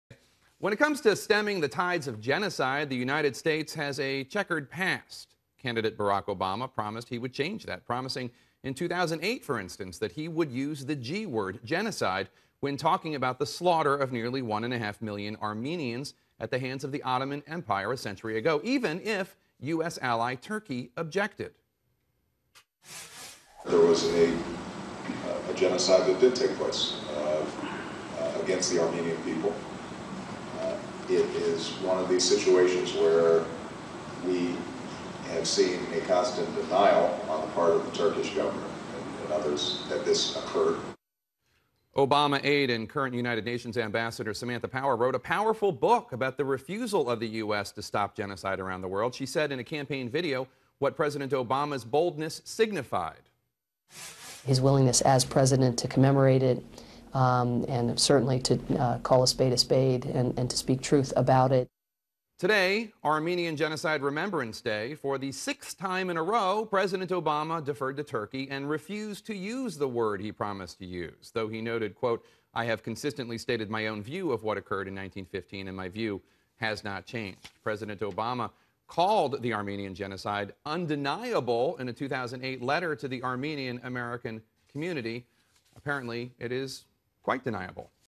CNN Slams Obama for Breaking Armenian Genocide Pledge
On April 24, 2014, CNN anchor and chief Washington correspondent Jake Tapper reports on President Obama's dramatic retreat from his pledge - for the sixth year in a row - to properly characterize the murder of 1.5 million Armenians as 'genocide.'

Using video of then Senator Obama, shared by the ANCA, and Samantha Power's campaign pitch for the President in 2008, Tapper concludes that the facts once characterized by Obama as undeniable, are in fact, for the President "quite deniable."